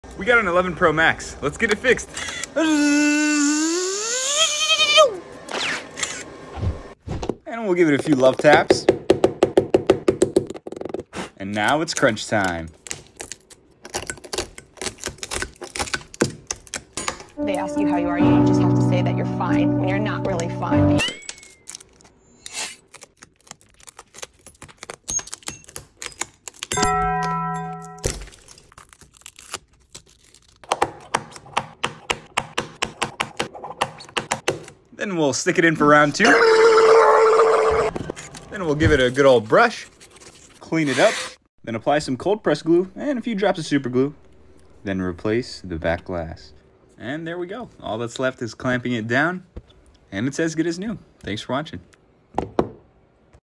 Another day at the office 🤠
Routine iPhone 11 Pro Max back glass repair. Hope you enjoyed!